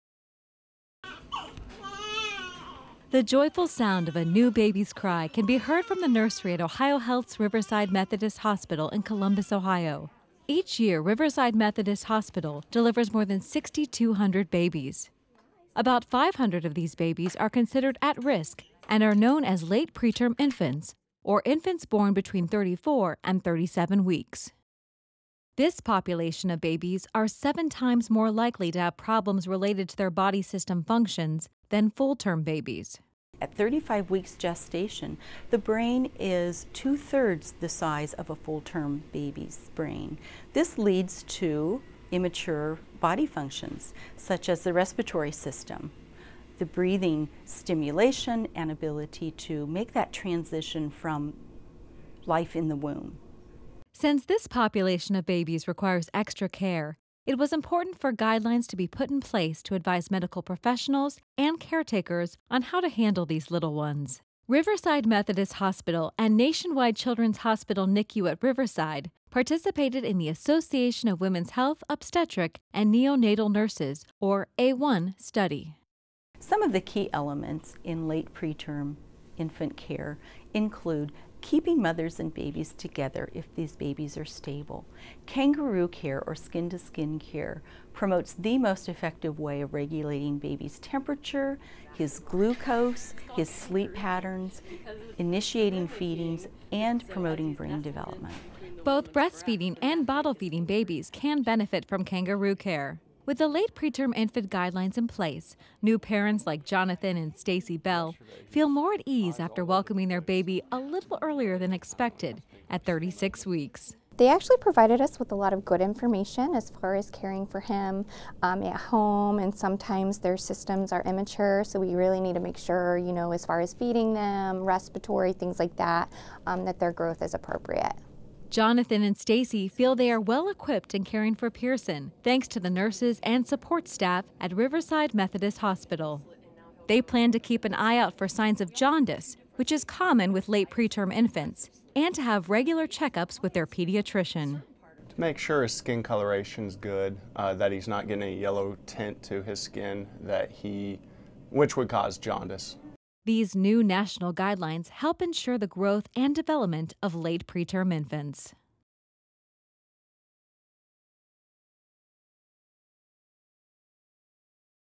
New Guidelines for Preemies: "Kangaroo Care" Benefits Early Infants
This fall, new guidelines were created for late preterm infants: babies born between 34 and 37 weeks. Nurses at Riverside Methodist Hospital, in Columbus, Ohio, helped shape those new guidelines, which include recommendations like "kangaroo care" or skin-to-skin contact between mother and child.